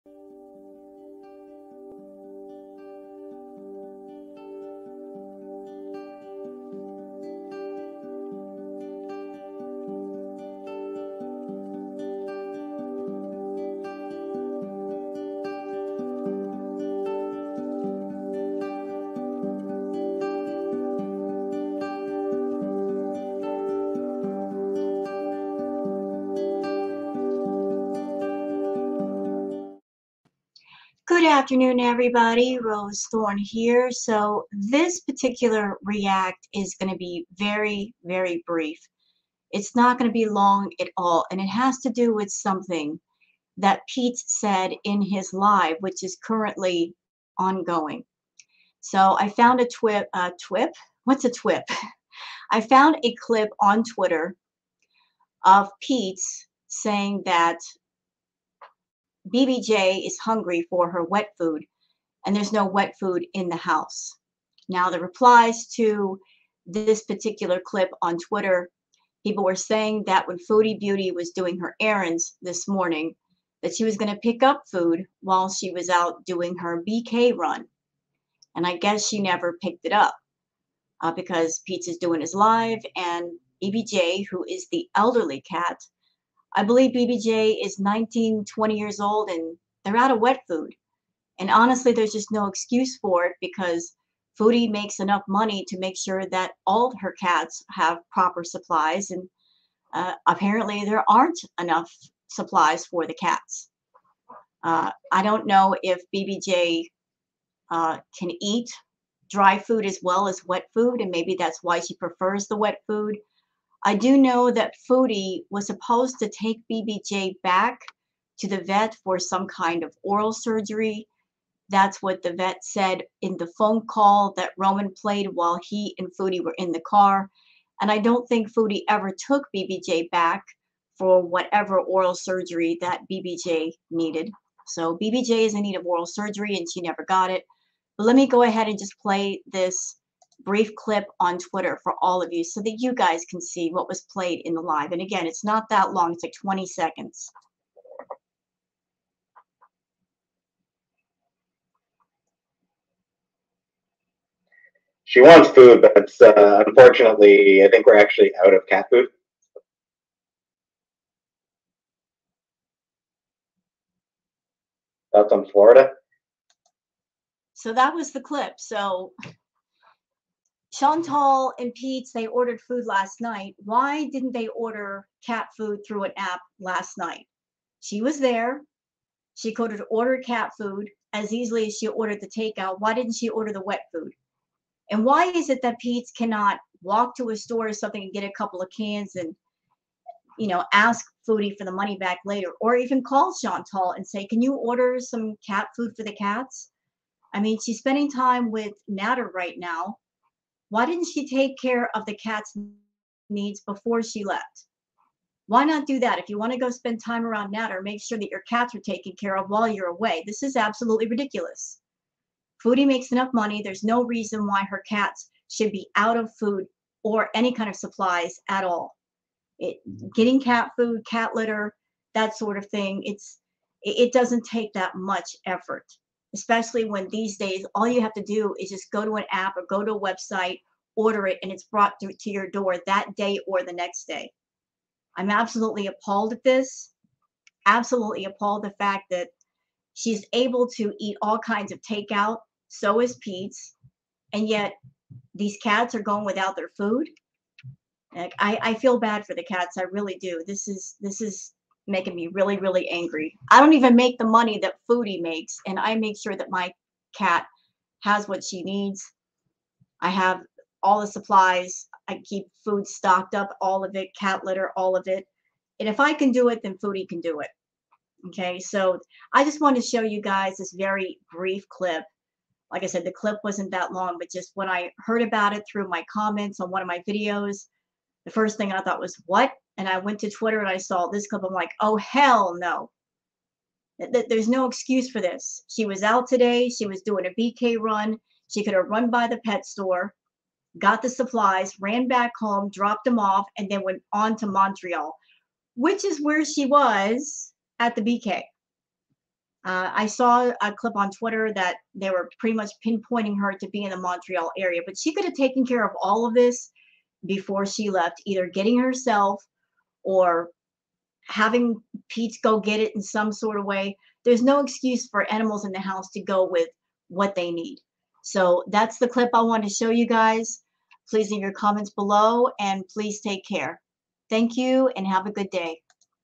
PEETZ SAYS BBJ IS OUT OF WET FOOD👎👎👎👎 ON LIVE REACT
So peetz revealed that chantal left peetz without wet cat food for bbj..who is an elderly cat with oral issues..i got some stuff to say about it...please watch and like and subscribe thanks